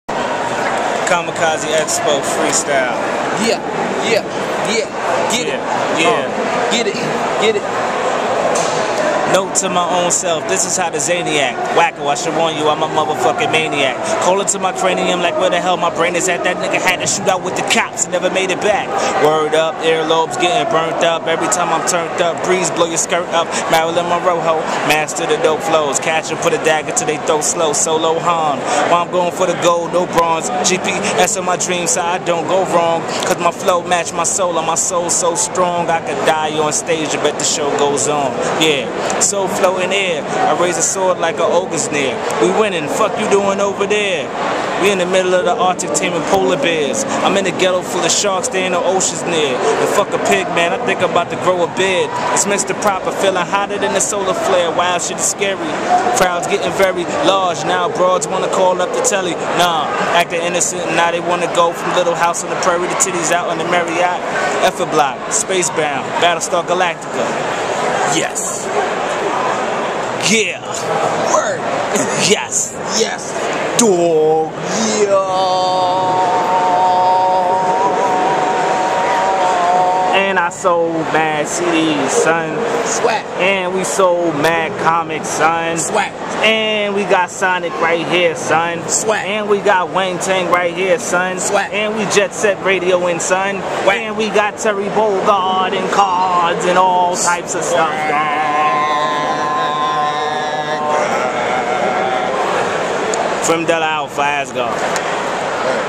Comikaze Expo Freestyle
So I was at the Comikaze Expo in LA selling CD's and decided to spit me a rhyme.. Enjoy